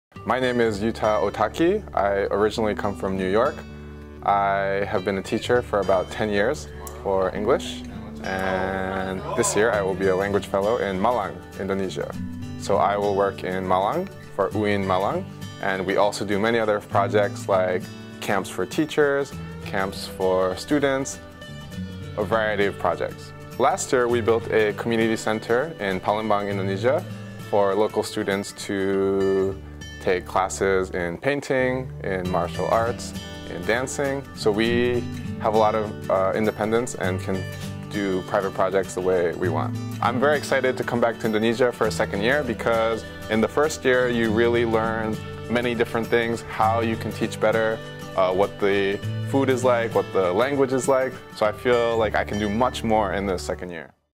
English Language Fellow Yuta Otake
The English Language (EL) Fellow Program in Indonesia promotes English language learning and fosters mutual understanding between the people of the United States and Indonesians. Meet Yuta Otake, the EL fellow in Malang.